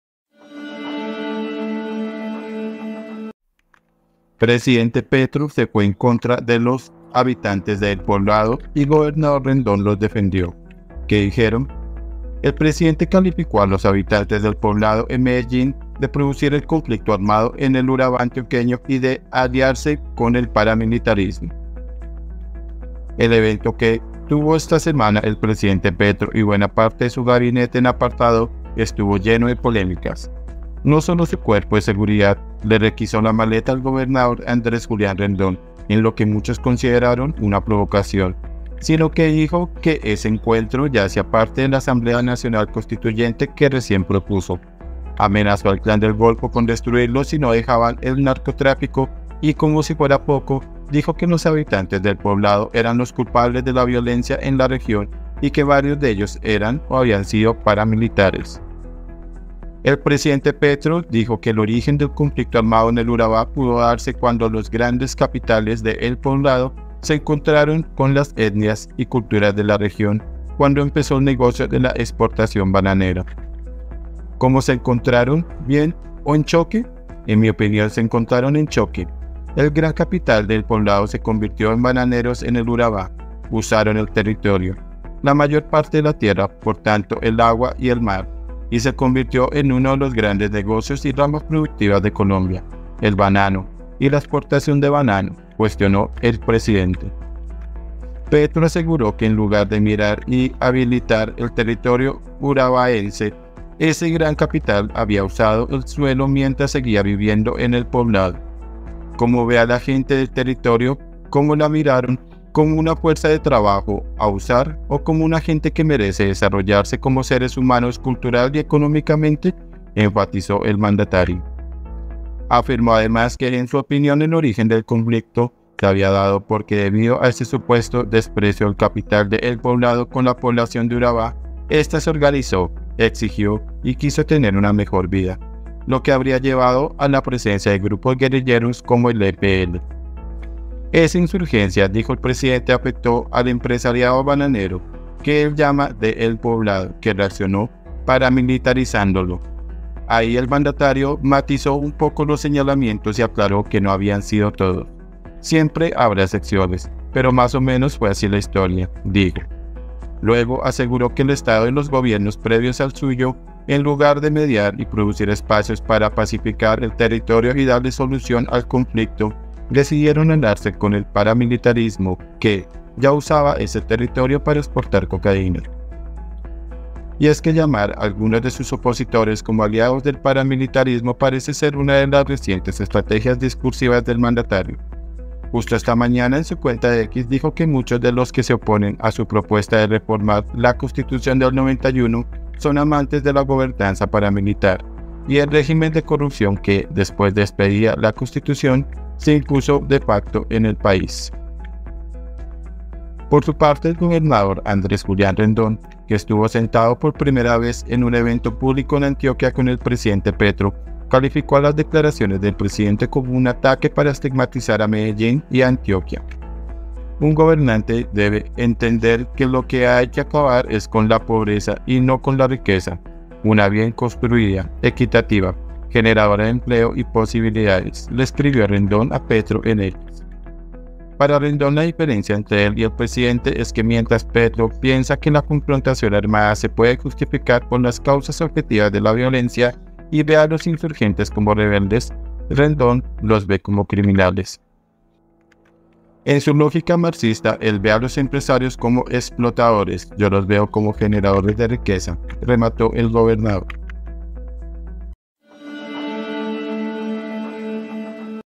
ATENCIÓN 🛑 PRESIDENTE PETRO SE VA EN CONTRA DE HABITANTES DEL POBLADO
Presidente Petro se fue en contra de los habitantes de El Poblado y gobernador Rendón los defendió, ¿qué dijeron?
El presidente calificó a los habitantes de El Poblado, en Medellín, de producir el conflicto armado en el Urabá antioqueño y de aliarse con el paramilitarismo-
EL COLOMBIANO